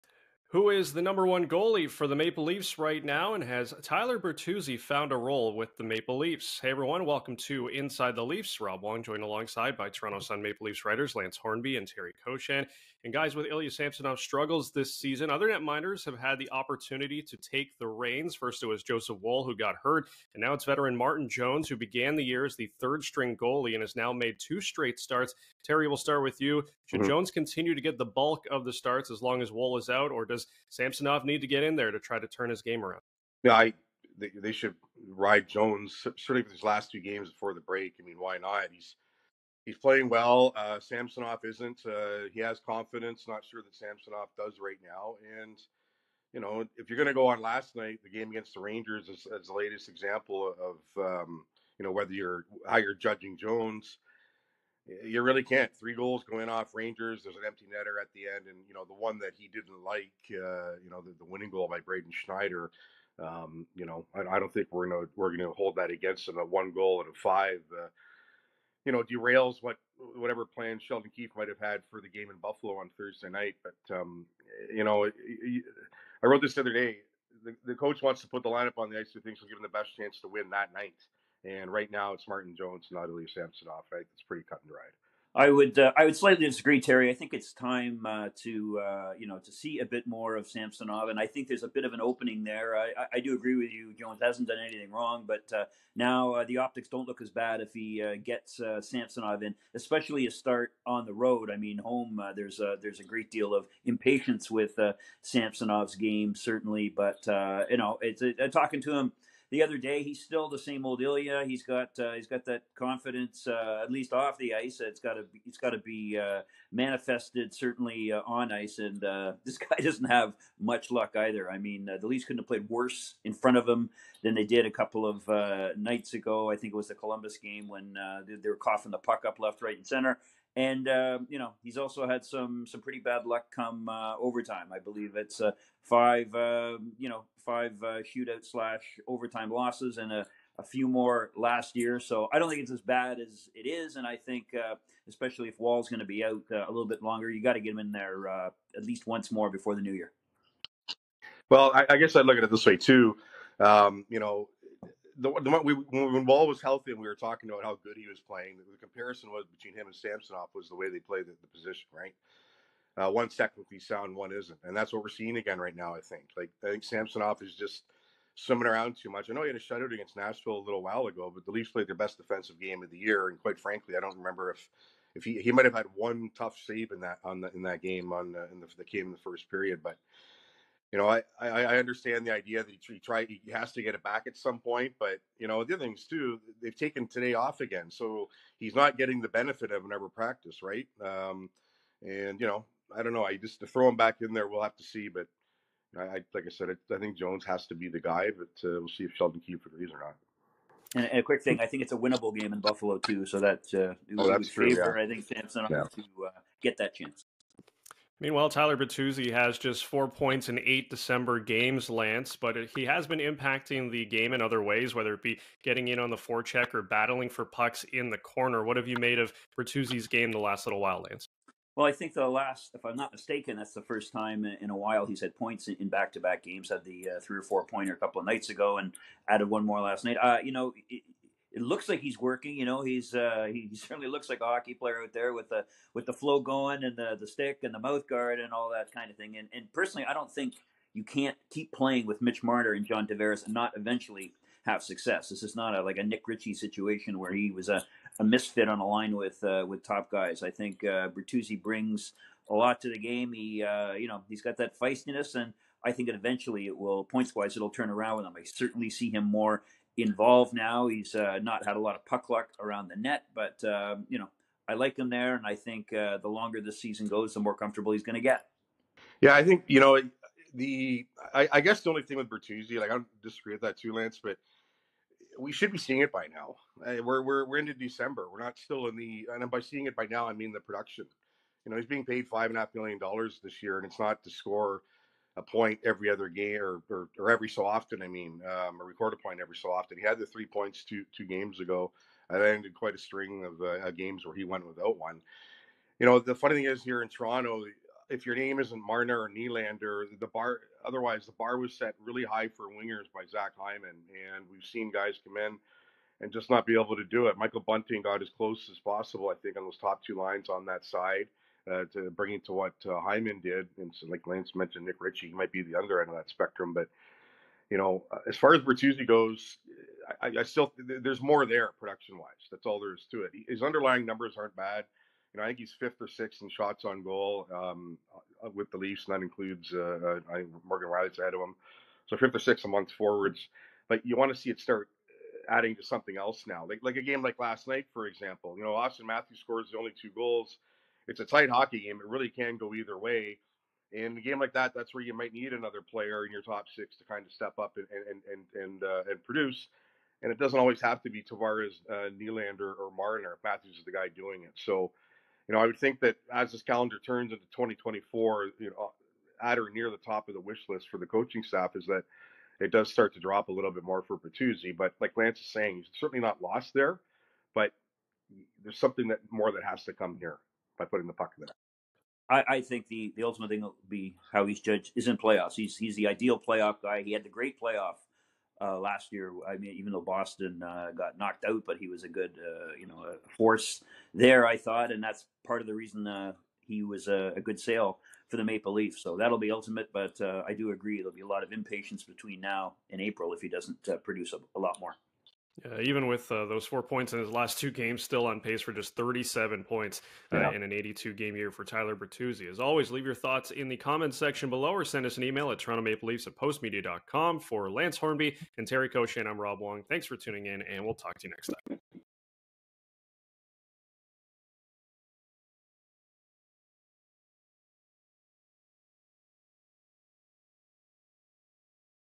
INSIDE THE LEAFS: Is Martin Jones the Leafs' No. 1 goalie?
On the latest edition of Inside The Leafs, Postmedia's Rob Wong chats with Toronto SUN Maple Leafs writers Lance Hornby and Terry Koshan about the Leafs' goaltending hierarchy with Martin Jones and Ilya Samsonov. They also discuss Tyler Bertuzzi's up-and-down season.